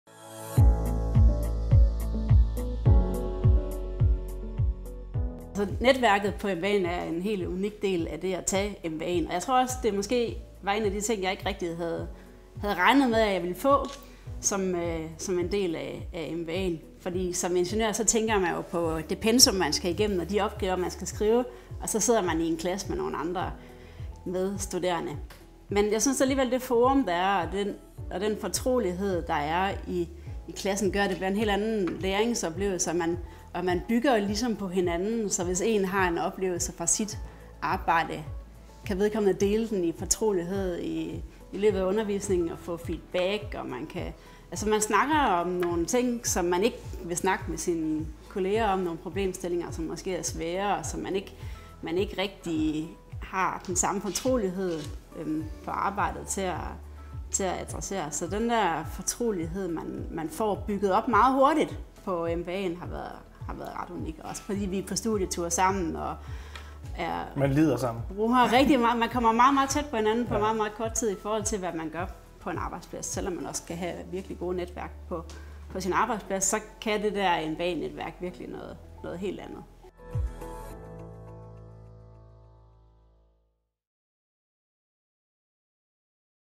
DTU Executive MBA: Ledelsesnetværket
EMBA-deltagere fra Danmarks Tekniske Universitet fortæller om deres erfaringer med DTU's Executive MBA.